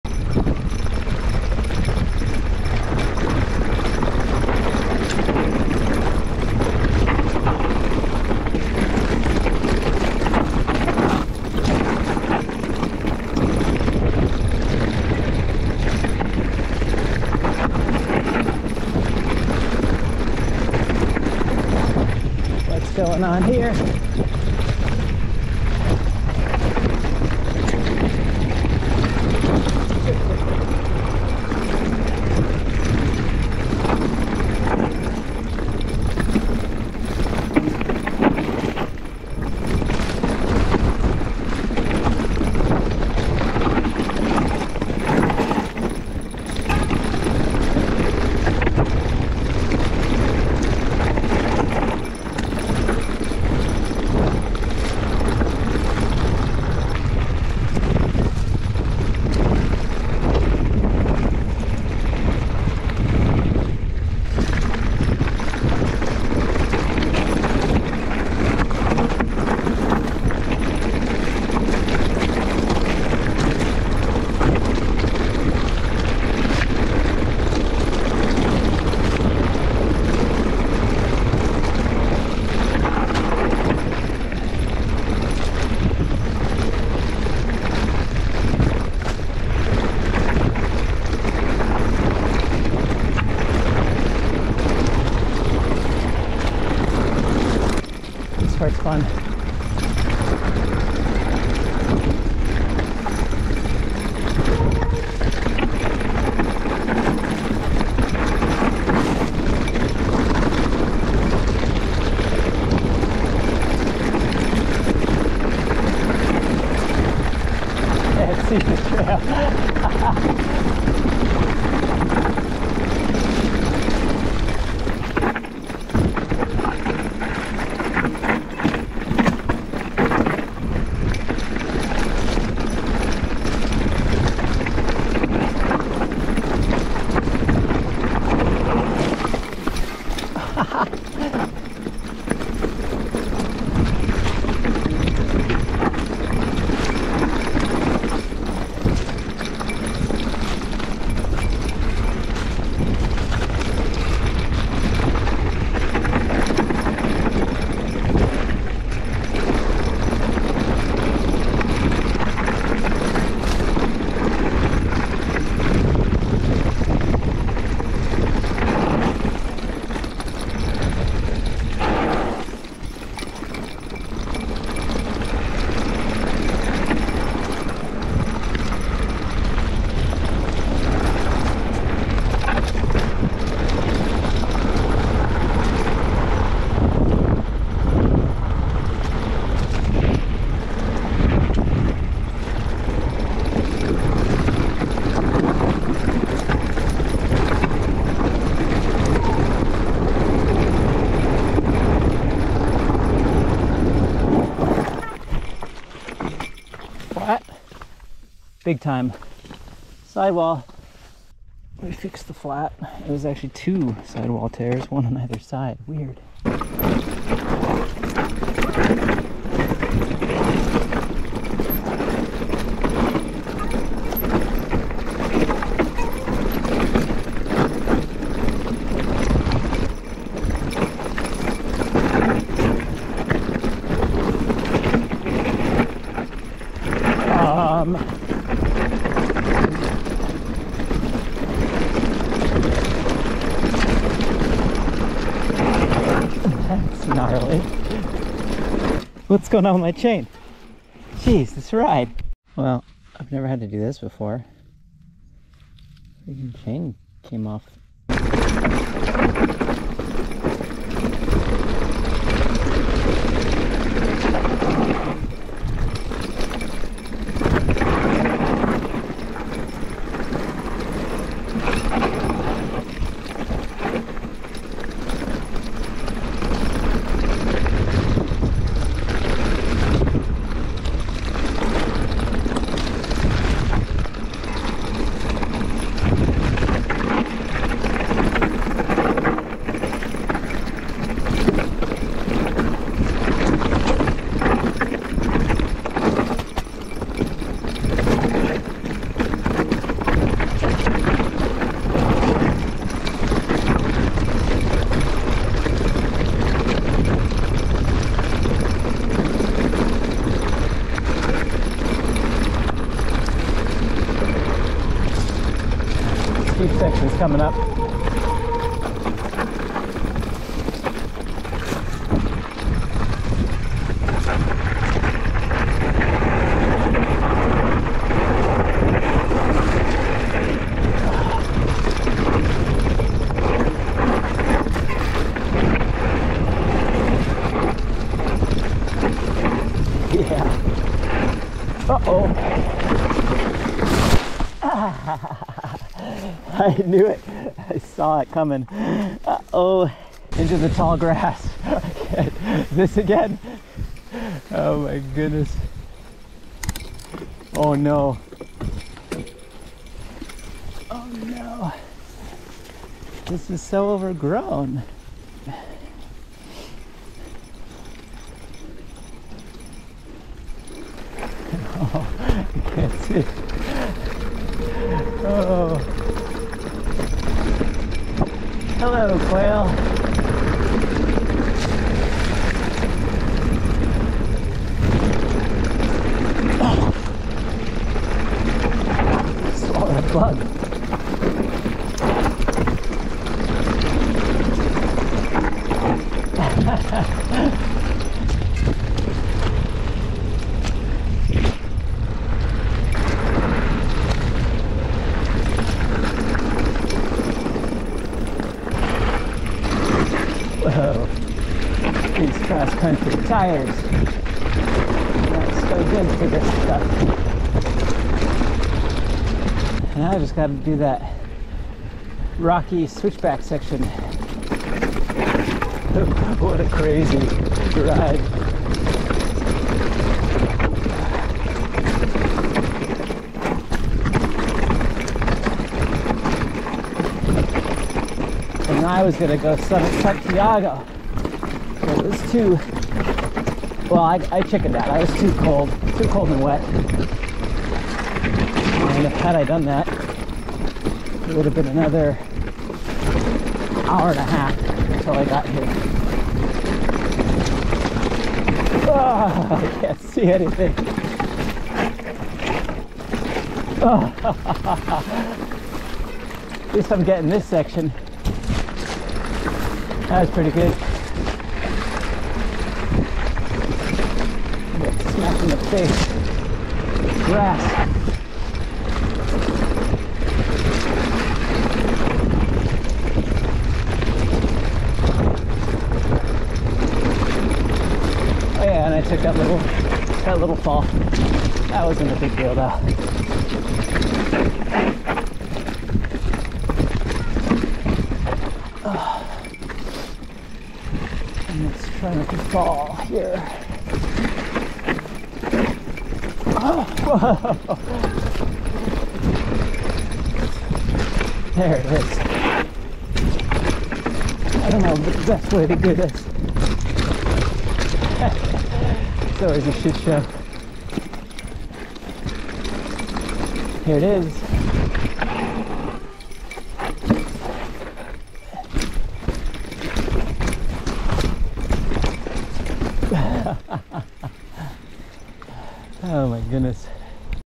Mountain Biking Los Pinos Ridge May 2023
This shows the condition of Los Pinos Ridge in May 2023, last year, after the long and record-breaking rainy season fueled the chaparral of our Southern California Santa Ana mountains, causing deep ruts and thick overgrowth on all our trails, especially our backcountry ones likes these.

This is the second chapter of a long ride in which I started at San Juan Trailhead, did a very cold, wet and bushy Chiquito trail to where it intersects the CA-74, then proceeded north on the rugged dirt road of Main Divide to Bear Spring (the water fill up spot at the intersection of Main Divide and Holy Jim), and finally doubled back to descend Los Pinos Ridge. The condition of the trail in this video is a little over grown, but not nearly as bad as Chiquito earlier in the day.